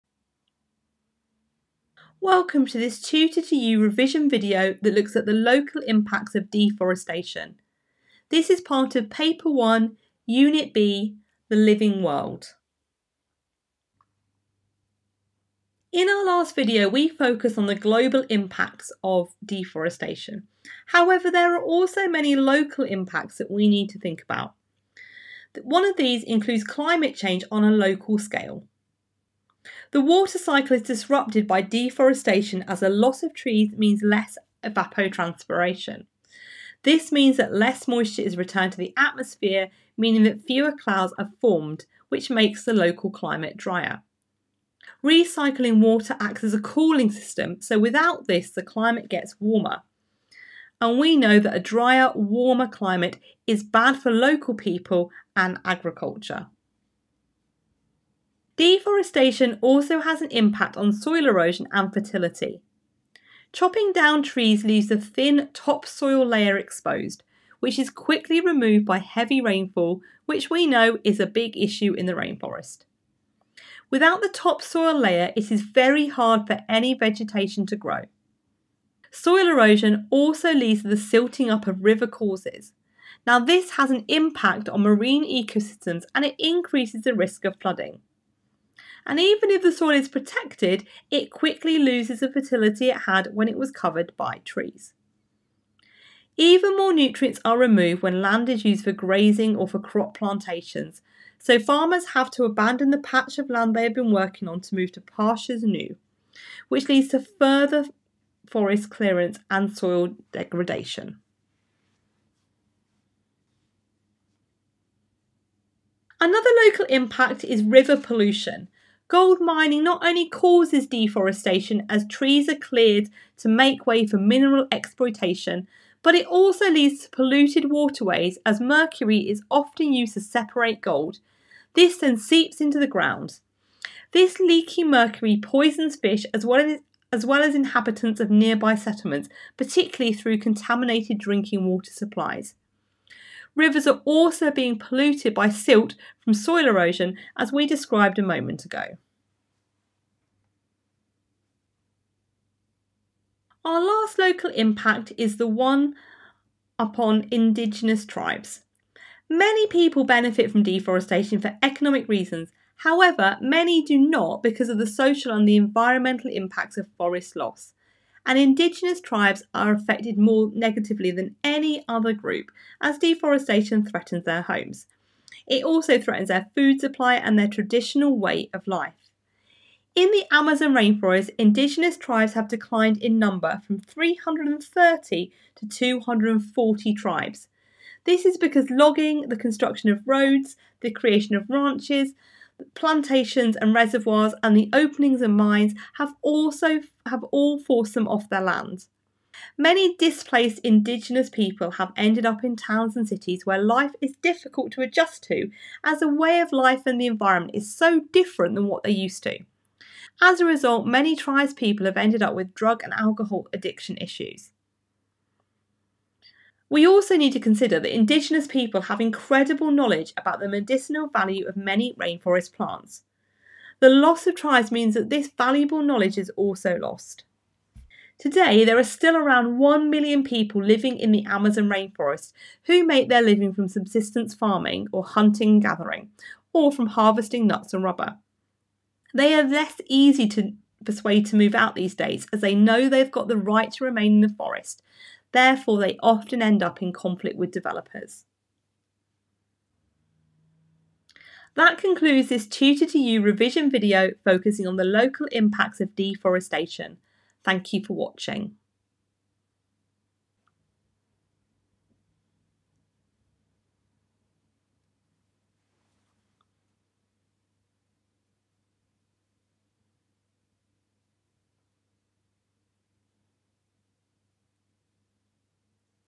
Local Impacts of Deforestation | AQA GCSE Geography | Tropical Rainforests 6
This topic video explores the different local impacts of deforestation, including local-scale climate change, soil erosion, river pollution (linked to mineral exploitation) and the effect on indigenous rainforest tribes.

This topic is part of the AQA GCSE Geography course - Paper 1: Unit 1B - The Living World.

Here is the playlist to use to study and revise all the topics for Paper 1: Unit 1B - The Living World:

VIDEO TIMESTAMPS
00:00 - Introduction
00:43 - Local impacts of deforestation on climate change (disruption of water cycle, drier climate, increased temperatures)
01:24 - Impact of deforestation on soil erosion and fertility (exposed topsoil, vegetation loss, soil degradation)
02:06 - River pollution due to gold mining (deforestation, mercury contamination, water pollution)
02:49 - Impact of deforestation on indigenous tribes (loss of homes, food supply, traditional way of life, decline in numbers, displacement, social and environmental impacts)
03:57 - Importance of indigenous knowledge and conflict with developers

VIDEO SUMMARY
This video focuses on the local impacts of deforestation in tropical rainforests. While the previous video explored the global impacts, it is crucial to understand the specific consequences at the local level. Deforestation disrupts the water cycle, leading to drier climates and reduced moisture in the atmosphere, negatively affecting local communities and agriculture. Soil erosion and fertility loss occur due to the removal of trees, leading to topsoil exposure and difficulty in vegetation growth. Gold mining contributes to deforestation and causes river pollution through the use of mercury, impacting both fish and nearby settlements. Indigenous tribes face severe consequences as their homes, food supply, and traditional way of life are threatened by deforestation, leading to displacement and loss of cultural knowledge. Lastly, conflicts arise between indigenous tribes and developers, highlighting the complex social and environmental dynamics.

KEY TERMS IN THE VIDEO
Local impacts of deforestation, Climate change, Water cycle disruption, Soil erosion, Soil fertility, River pollution, Gold mining, Indigenous tribes, Cultural loss, Environmental conflicts, Rainforest communities.